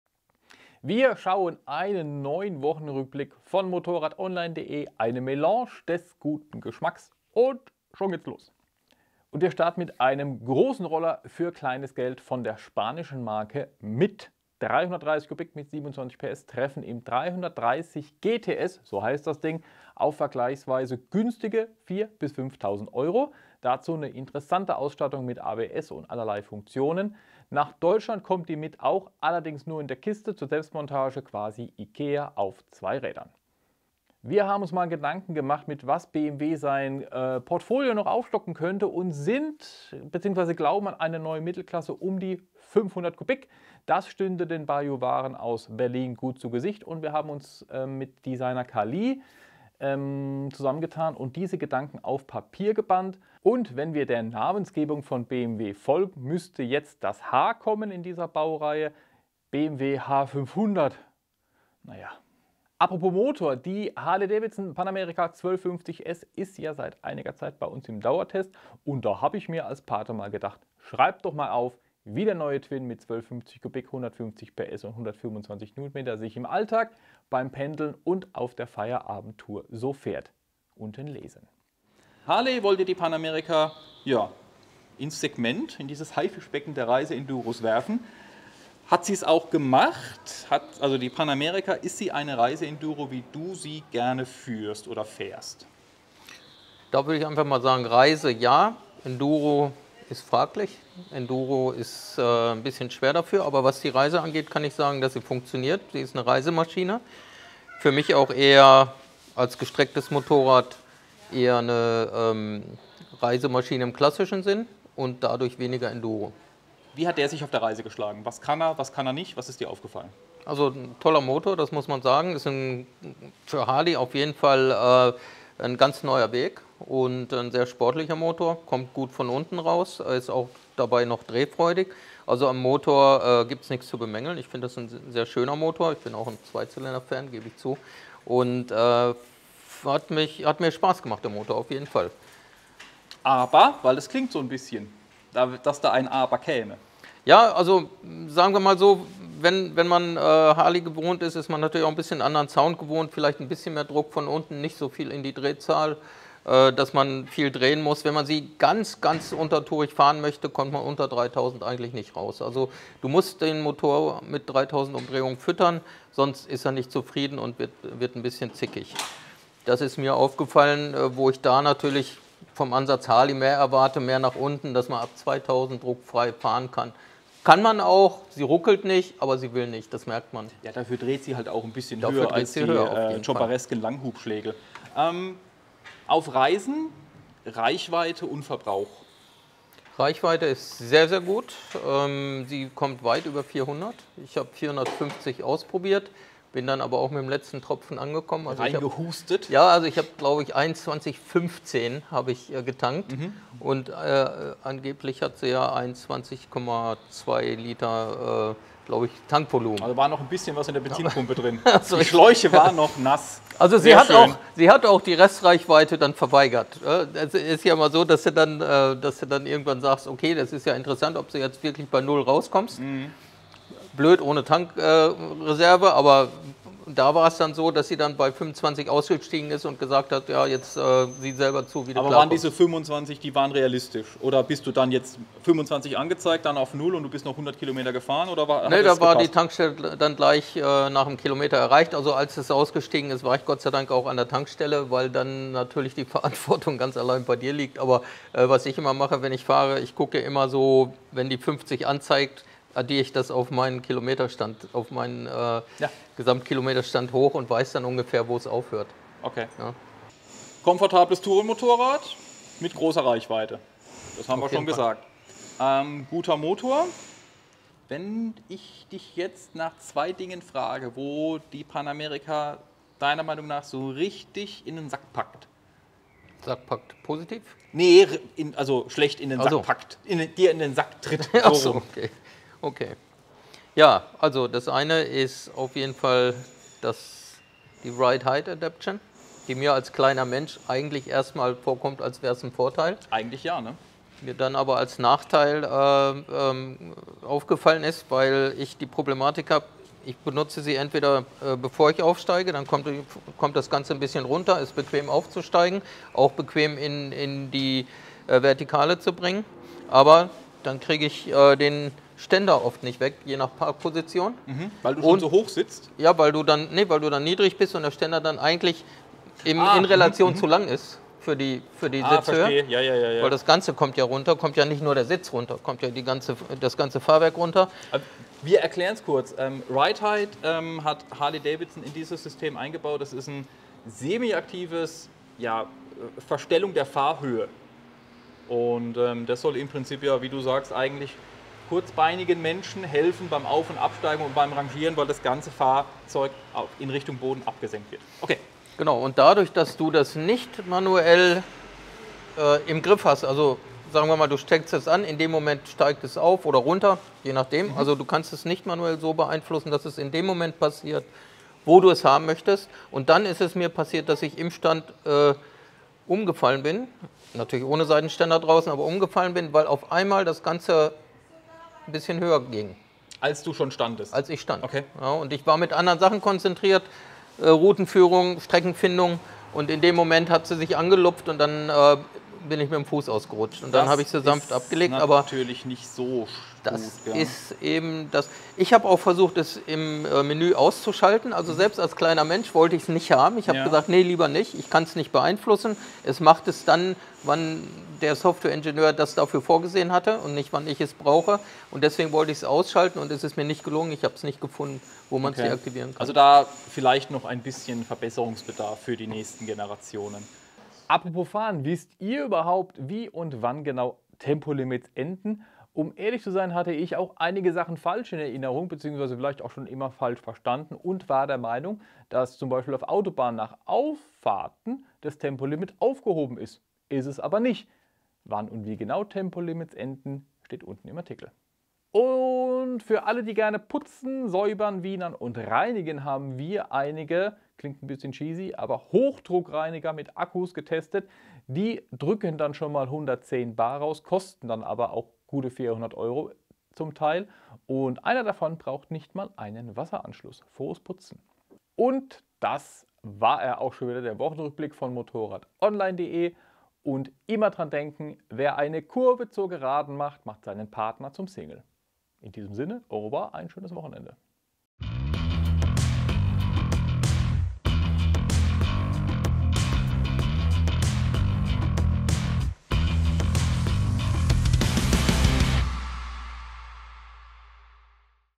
Harley-Davidson Pan America im Dauertest – MOTORRAD Die Woche im Überblick #43 27.08.-02.09.
Wieder ist eine Woche vorbei. Welche Themen auf MOTORRAD Online das Interesse weckte, zeigen wir in der Wochenübersicht.